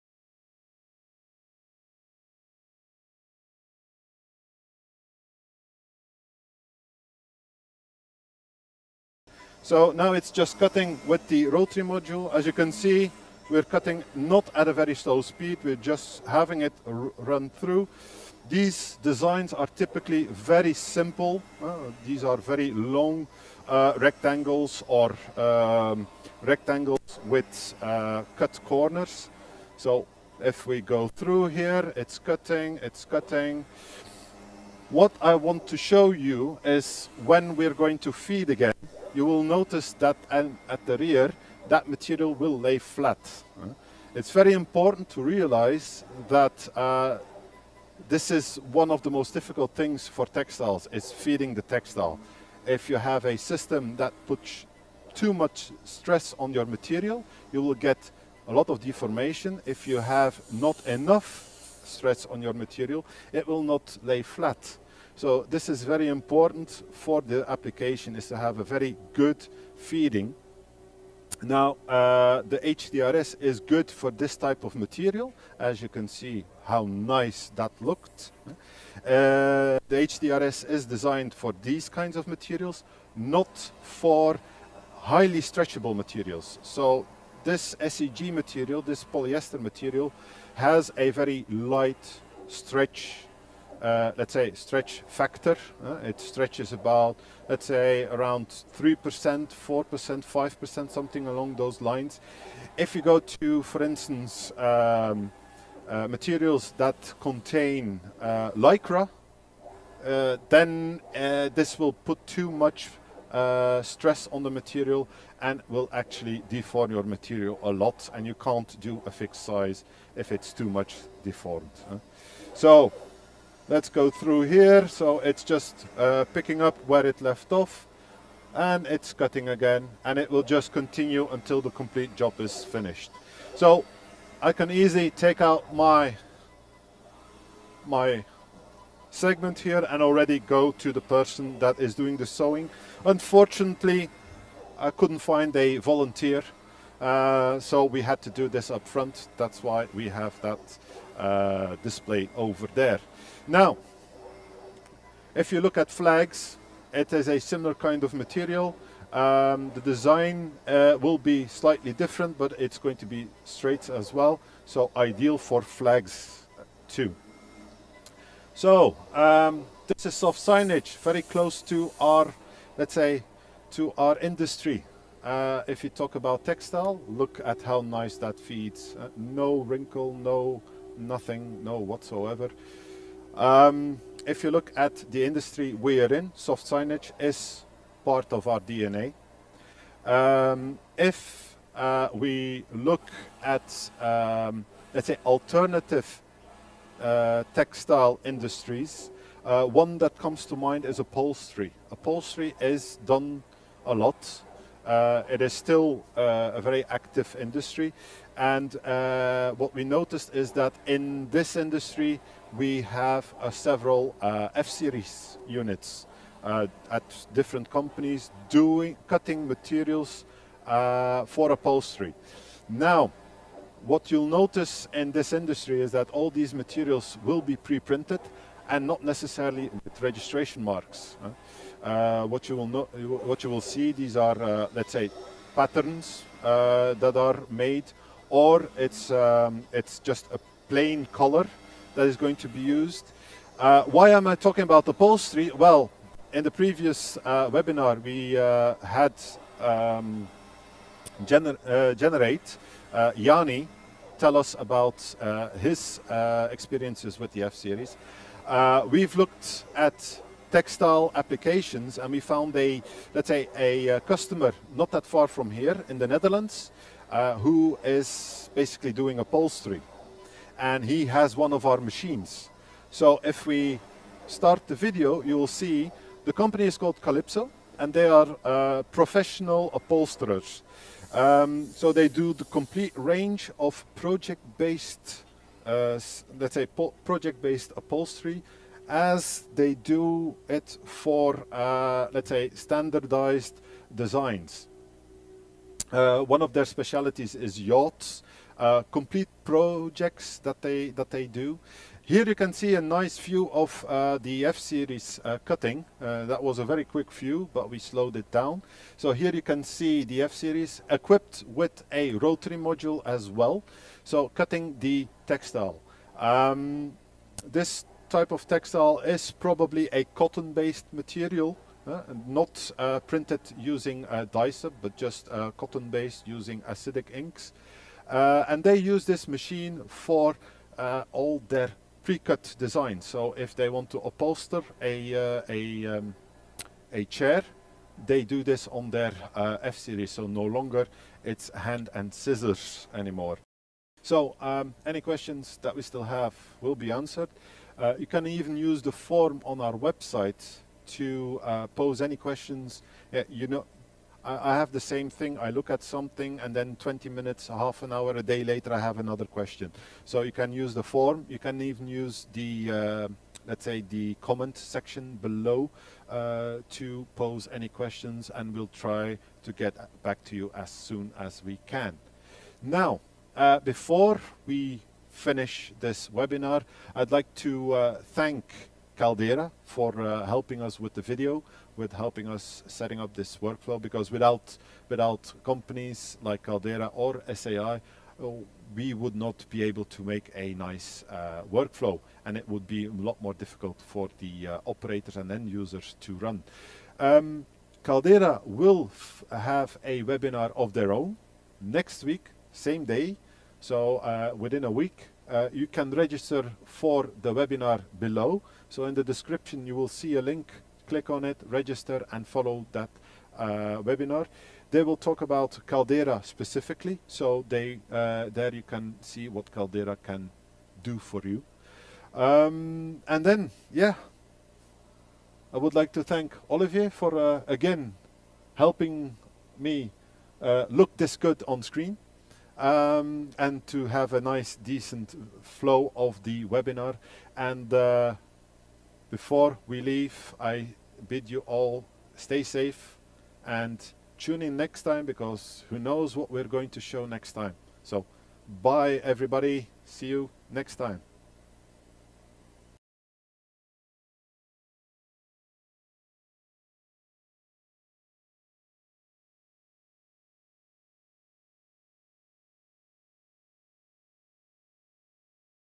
Summa F - cięcie tekstyliów
Cięcie tekstyliów za pomocą Summa F